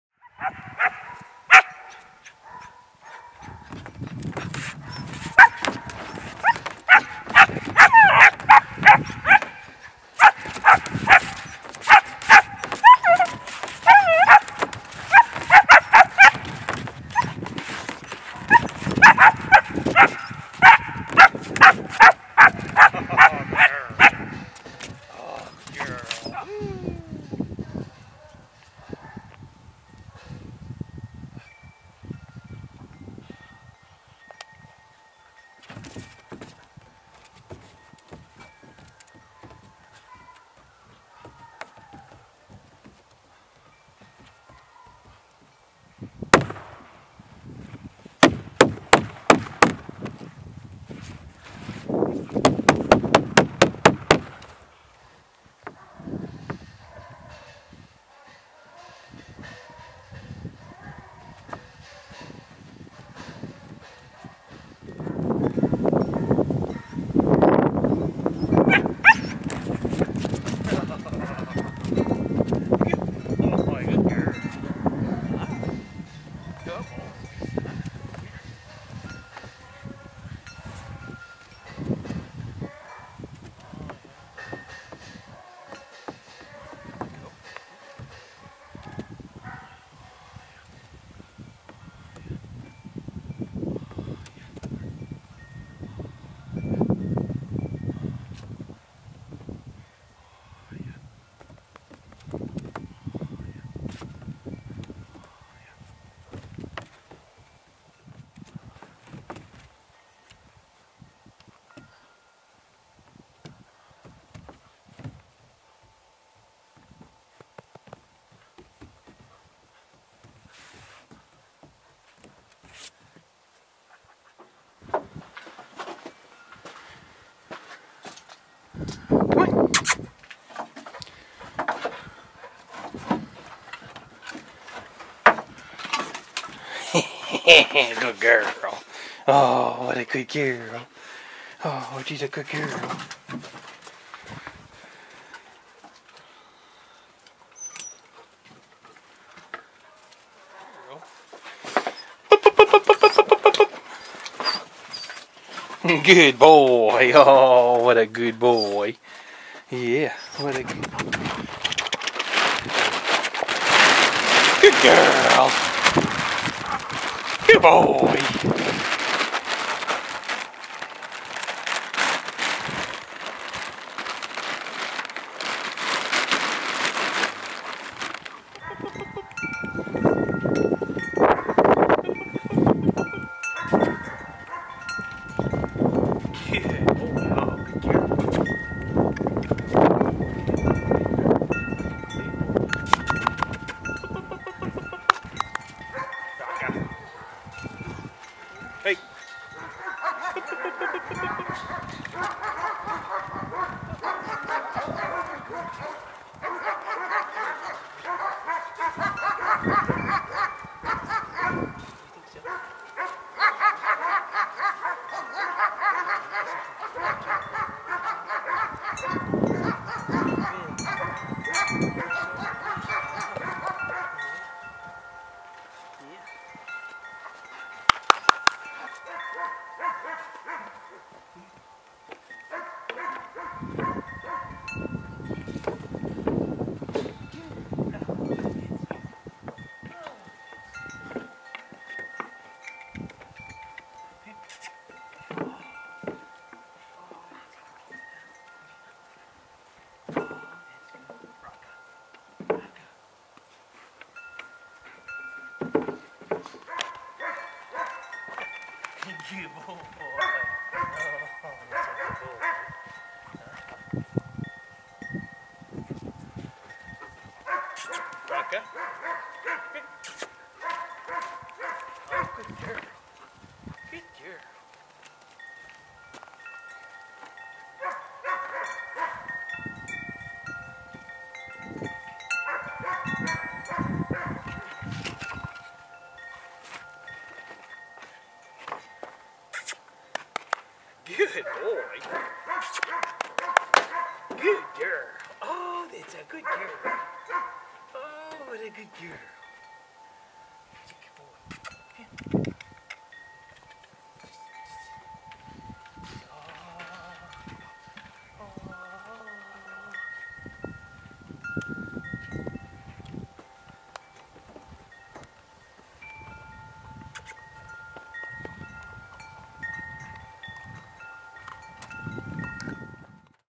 German Shepherd K9 Pups
Two 3 month old German Shepherd pups from Von Woden Kennels in training as potential police K9s.  Solid drives, excellent health.